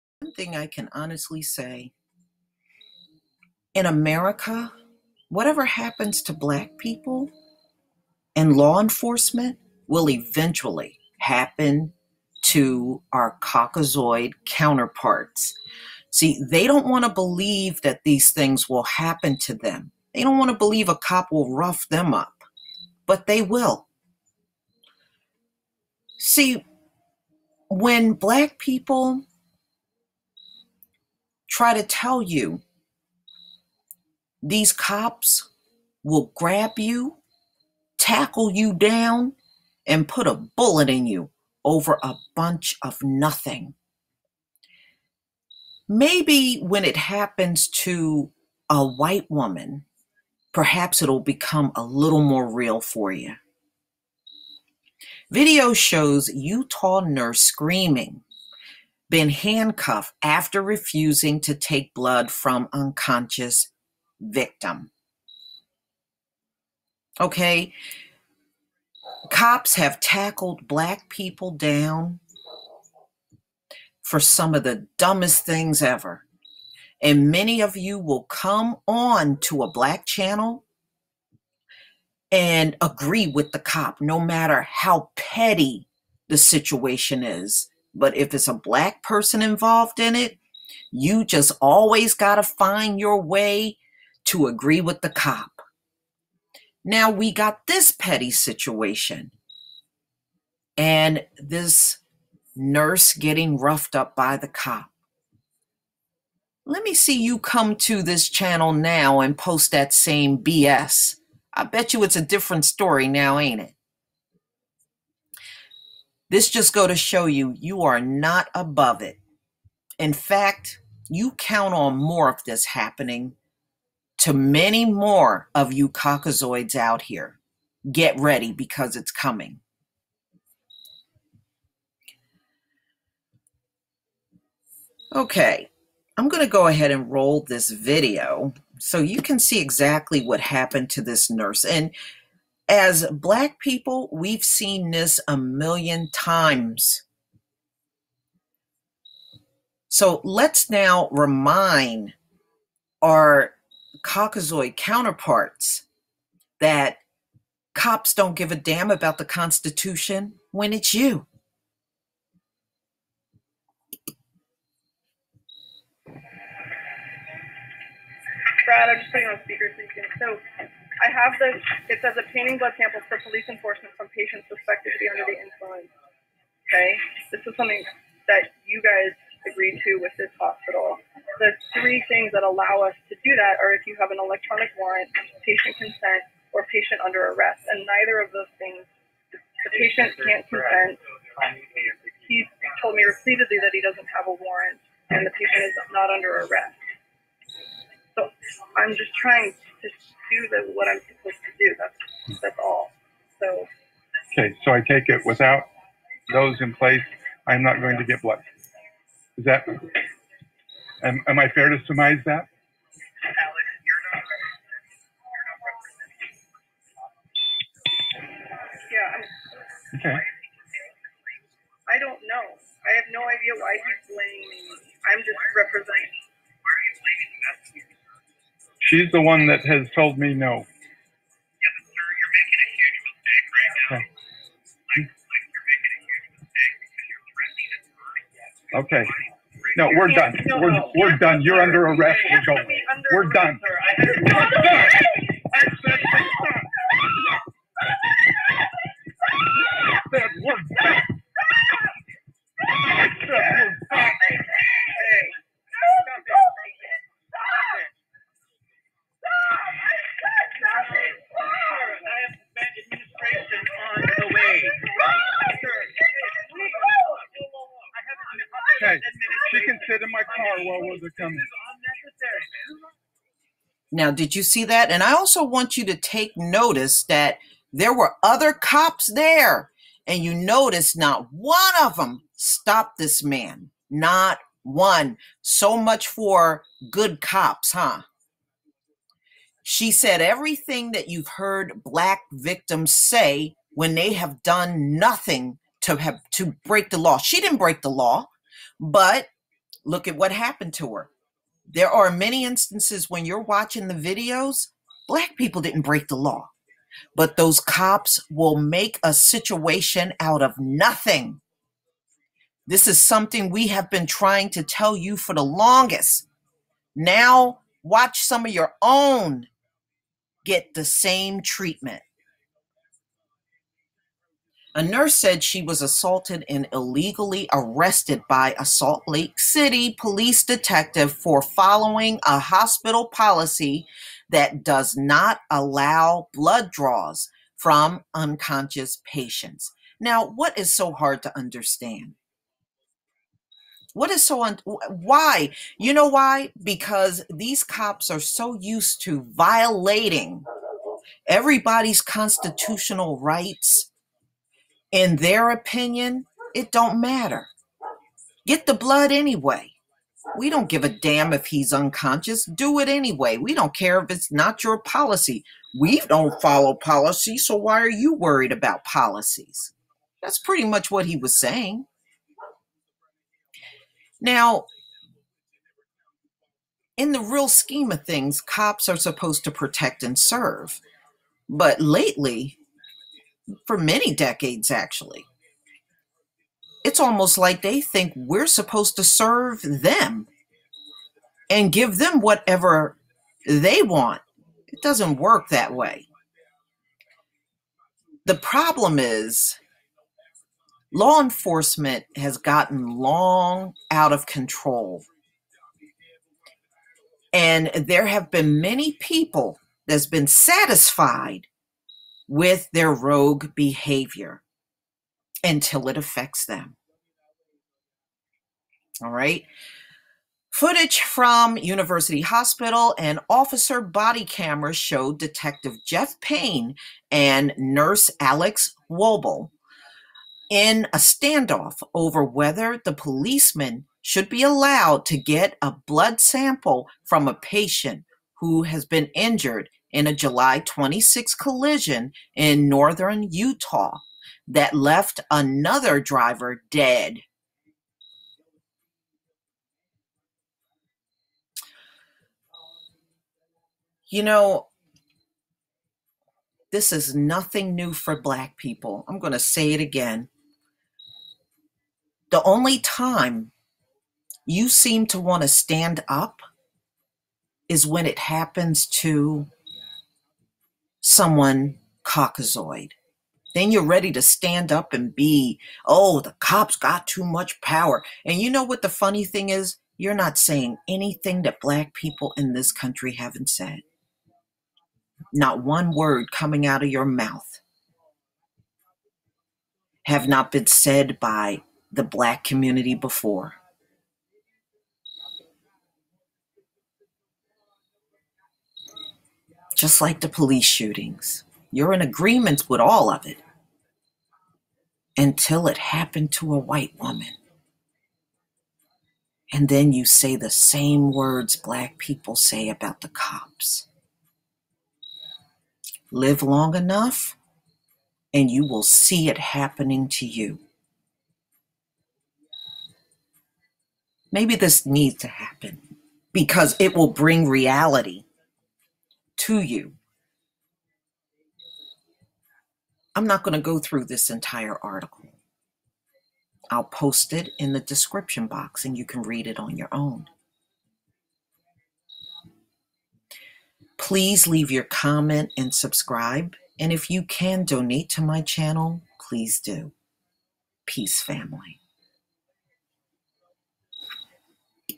White Nurse Ruffed Up and Arrested For Following Hospital Policy In Utah
Video shows Utah nurse screaming, being handcuffed after refusing to take blood from unconscious victim